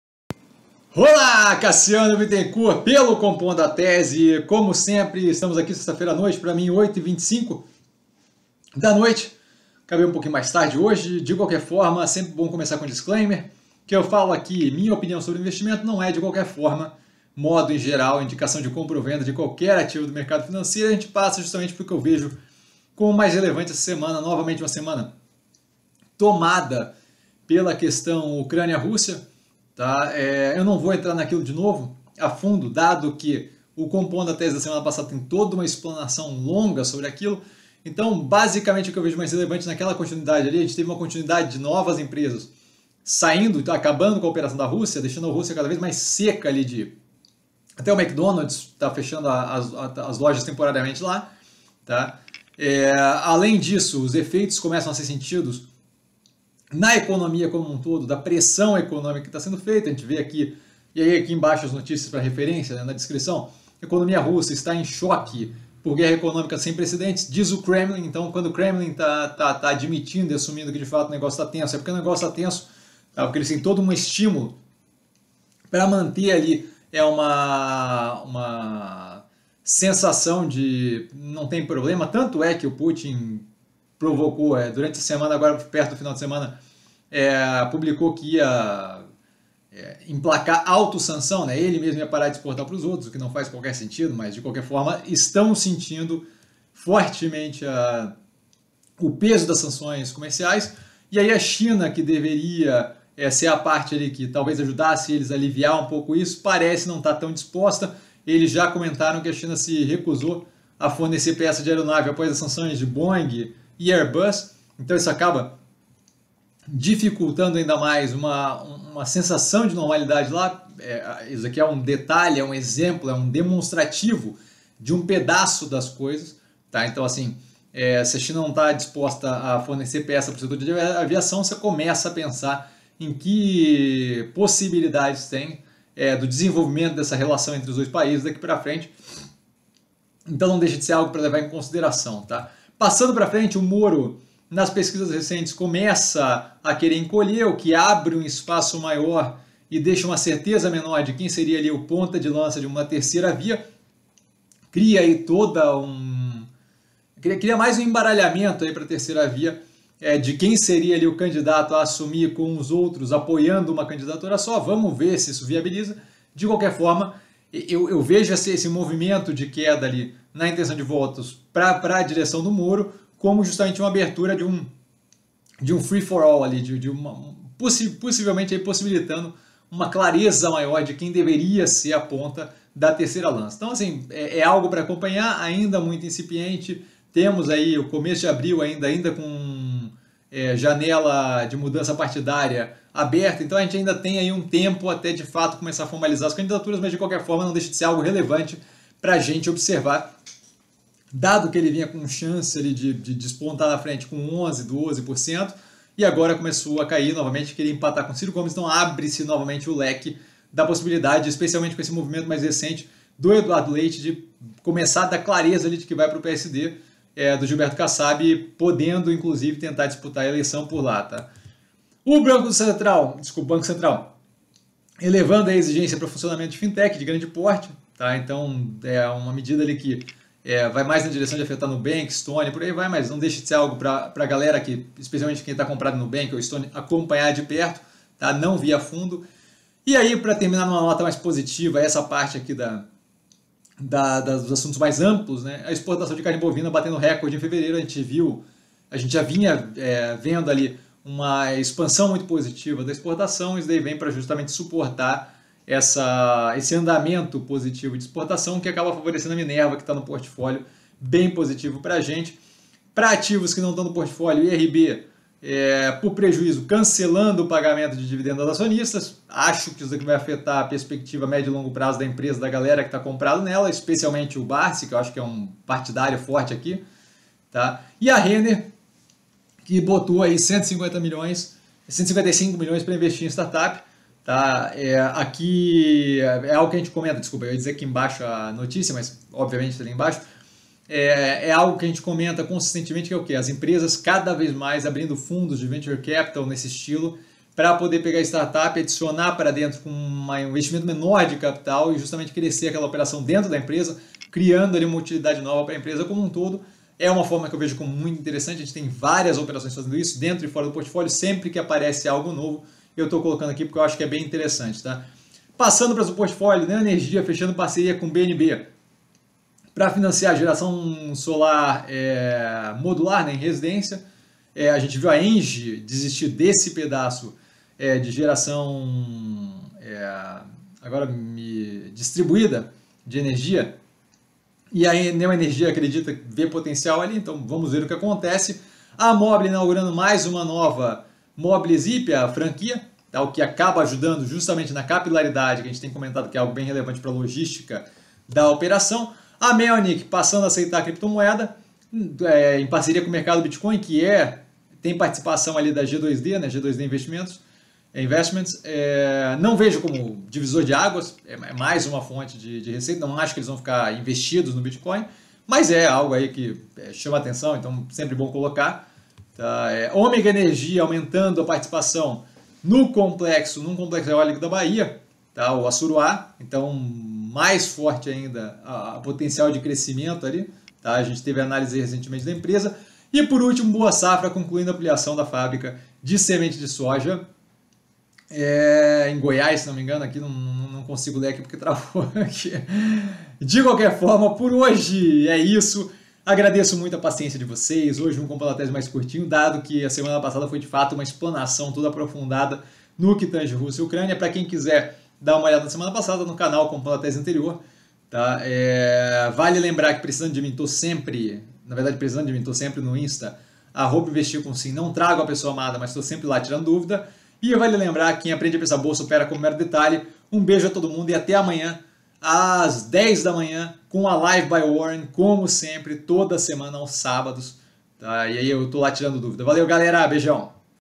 11.03.2022 - Compondo a Tese - 04/03/22 a 11/03/22
Economia russa está em "choque" por guerra econômica sem precedentes, diz Kremlin

Rússia diz que China se recusa a fornecer peças de aeronaves após sanções

BC eleva exigência para funcionamento de fintechs de maior porte

Por prejuízos, IRB (IRBR3) cancela pagamento de dividendos a acionistas

Renner terá R$ 155 milhões para investir em startups; CEO detalha os planos do fundo

Neoenergia fecha parceria com BNB para financiamento de geração solar em residências

Mobly (MBLY3) inaugura nova loja Mobly Zip em Indaiatuba, em São Paulo

Construtora Melnick (MELK3) passa a aceitar criptomoedas, após parceria com Mercado Bitcoin

Omega Energia aumenta participação em complexo eólico na Bahia

Boa Safra conclui ampliação de fábrica de sementes de soja em Goiás